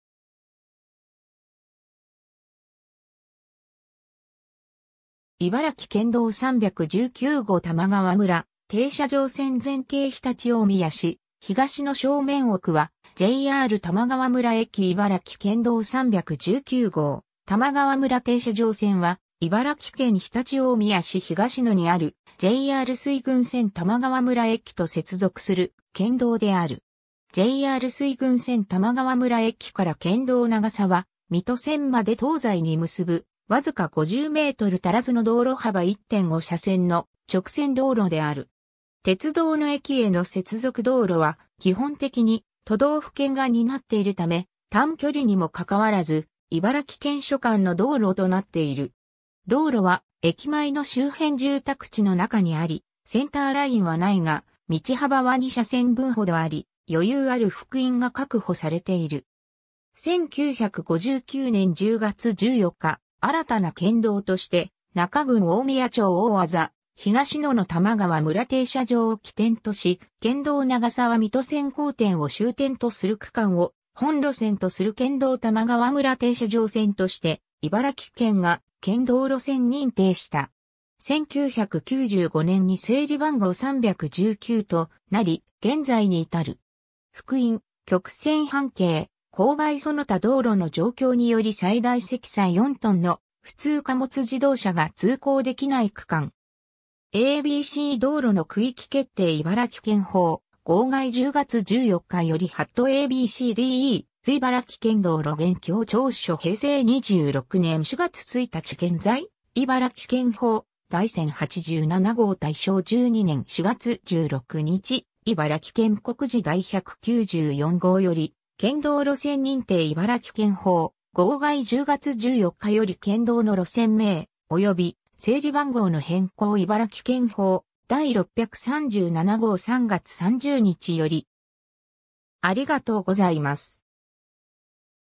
茨城県道319号玉川村停車場線
茨城県道319号玉川村停車場線 全景 常陸大宮市東野（2014年6月） 正面奥はJR玉川村駅 茨城県道319号玉川村停車場線（いばらきけんどう319ごう たまがわむらていしゃじょうせん）は、茨城県常陸大宮市東野にあるJR水郡線玉川村駅と接続する県道である。
 JR水郡線玉川村駅から県道長沢水戸線まで東西に結ぶ、わずか50m足らずの道路幅1.5車線の直線道路である。
鉄道の駅（近年建設された駅や停留所と呼ばれる一部無人駅を除く）への接続道路は基本的に都道府県が担っているため、短距離にもかかわらず茨城県所管の道路となっている。
道路は駅前の周辺住宅地の中にあり、センターラインは無いが道幅は2車線分程あり、余裕ある幅員が確保されている。
 1959年（昭和34年）10月14日、新たな県道として那珂郡大宮町大字東野の玉川村停車場を起点とし、県道長沢水戸線交点を終点とする区間を本路線とする県道玉川村停車場線として茨城県が県道路線認定した。
 1995年（平成7年）に整理番号319となり現在に至る。
 ^ a b c 道路の区域決定（昭和三十四年十月十四日 茨城県告示第九百三号） (PDF) ,茨城県報 号外（1959年（昭和34年）10月14日）より ^ a b c d e 『茨城県道路現況調書』平成26年4月1日現在 ^ 茨城県報　第千八十七号　大正十二年四月十六日 - 茨城県告示第百九十四号より ^ 県道路線認定（昭和三十四年十月十四日 茨城県告示第九百一号） (PDF) ,茨城県報 号外（1959年（昭和34年）10月14日）より ^ 県道の路線名および整理番号の変更（平成7年3月30日 茨城県告示第436号） (PDF) ,茨城県報 第637号（1995年（平成7年）3月30日）より